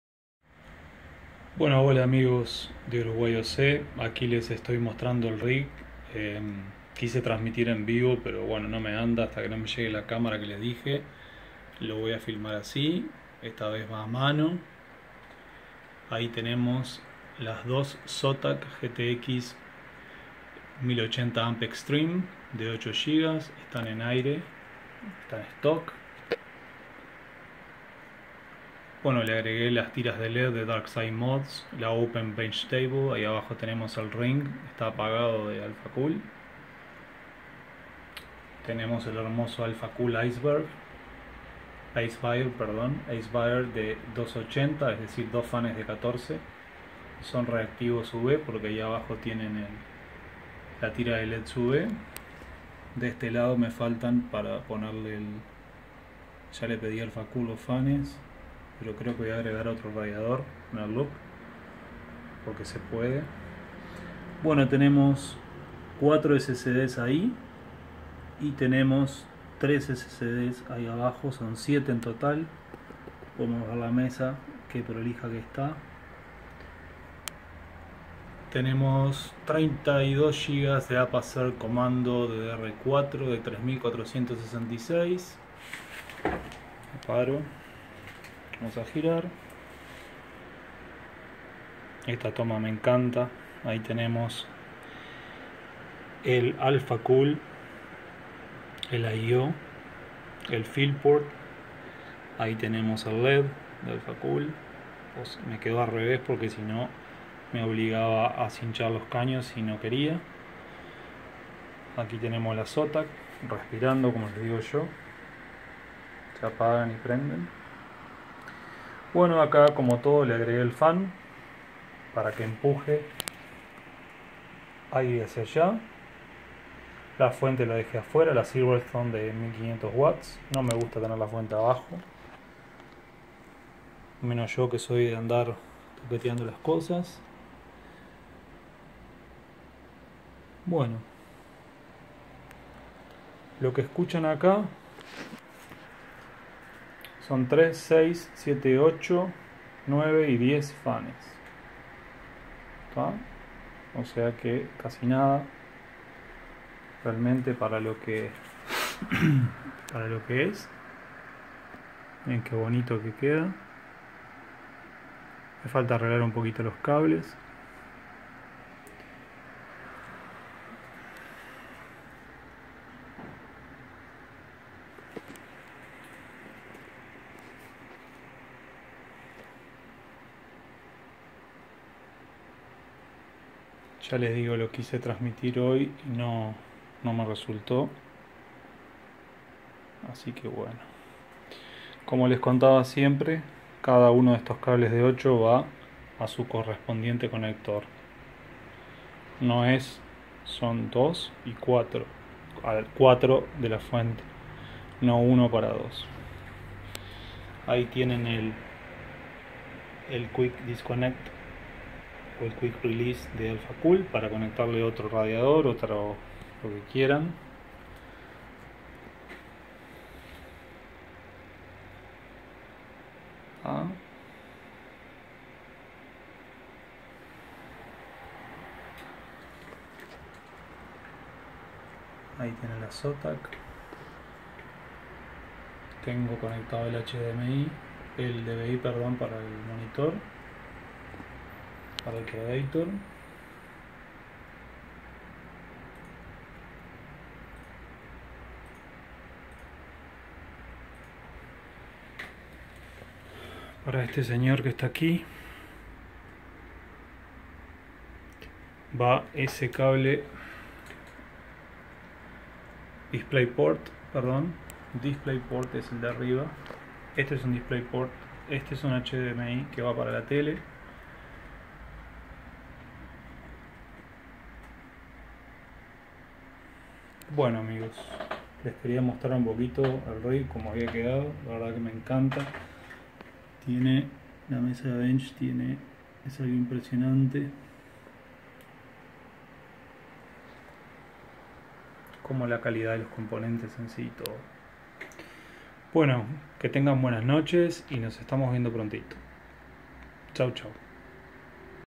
Una Vista Nocturna al Proyecto OBT
Intel I5960X + 32GB DDR4 3200C16
ASRock X99 OC Formula 3.1
1x 512GB NVMe SSD, OS y Tools
2x ZOTAC GTX 1080AMP! Extreme SLI
Cooled by Alphacool
Powered by Silverstone ST500W GS Gold PSU
Open Bench Table

Hardware, Overclocking, Gaming, Water Cooling y Nuevas Tecnologías